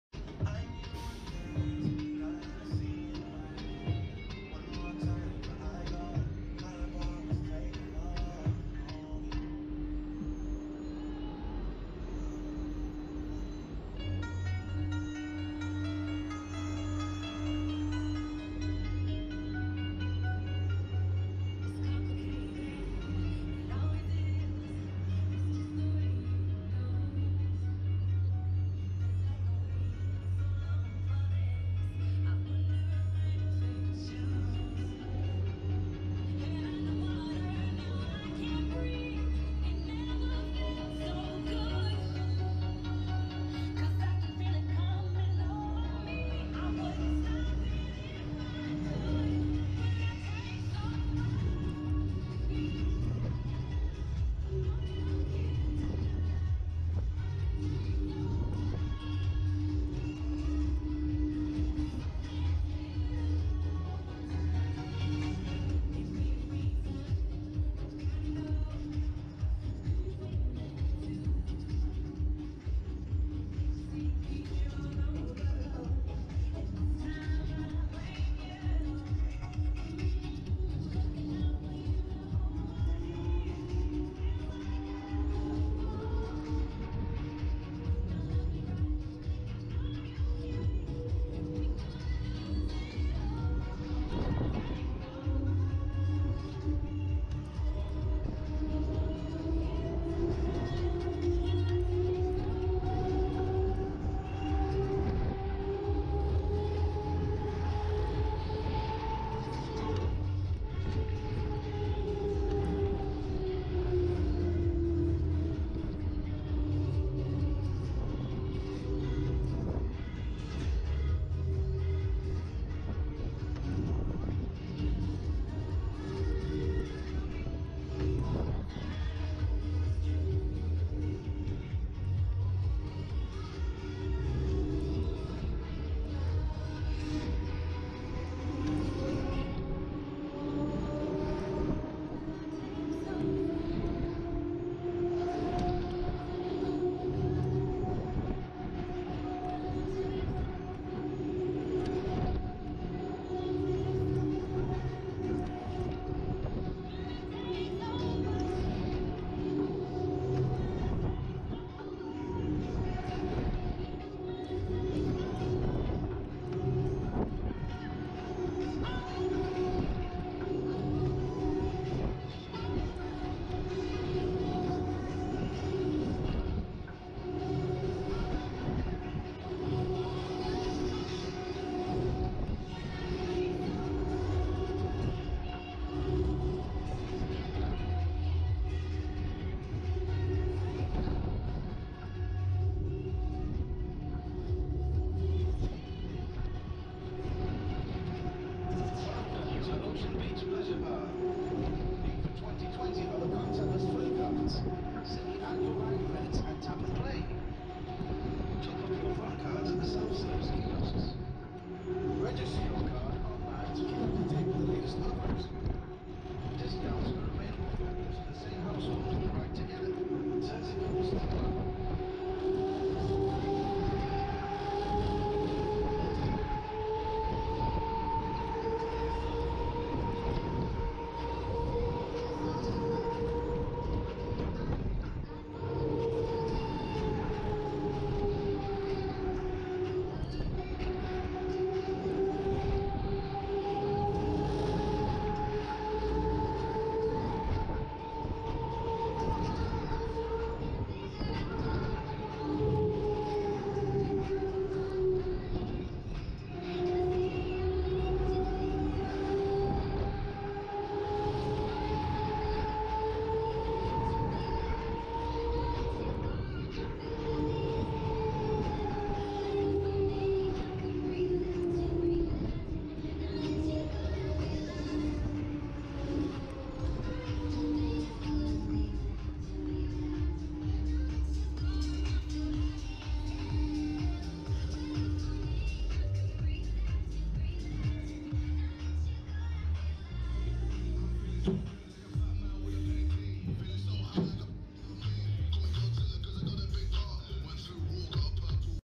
Break Dance (William Houghton) On Ride POV Ocean Beach Pleasure Park 2020
Huss Breakdance